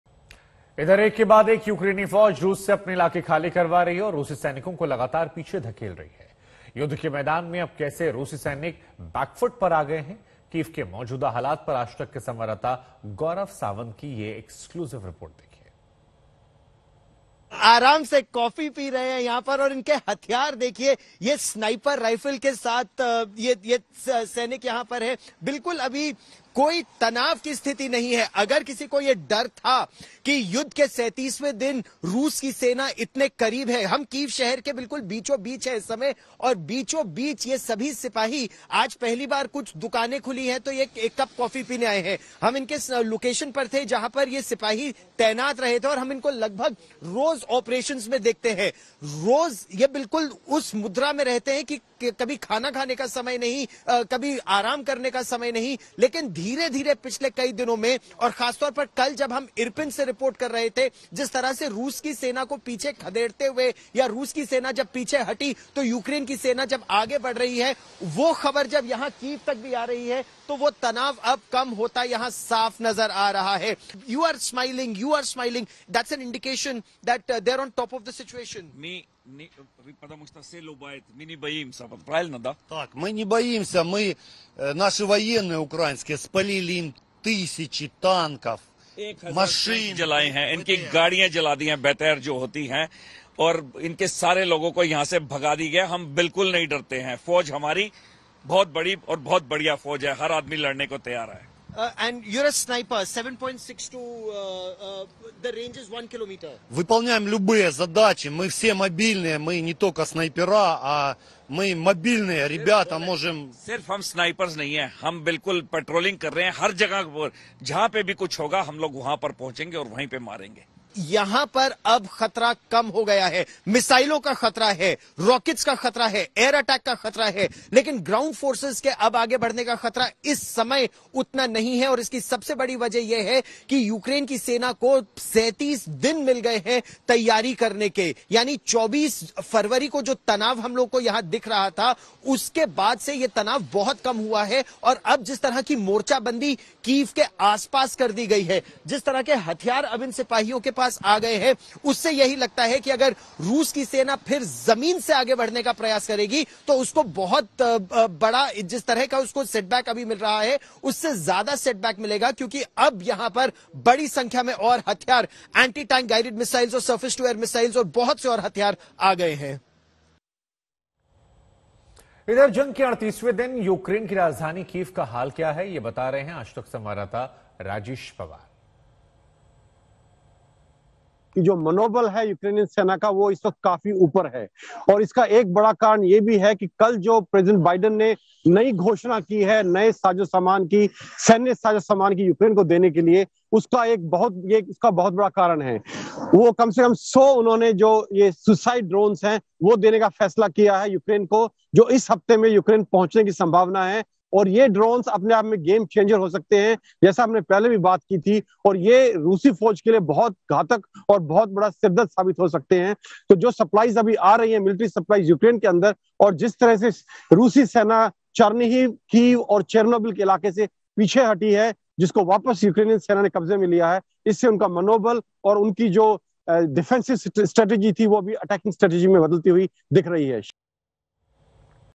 रुस से अपने इलाके खाली करवा रहा Ukraine, देखें यूक्रेनी सैनिकों के साथ Gaurav Sawant की रिपोर्ट| War
Russia invaded Ukraine in a pre-dawn attack on February 24 and from early on, Russian troops and artillery have been trying to enter Kyiv. Although they managed to come dangerously close to the capital city, they could not breach the city limits.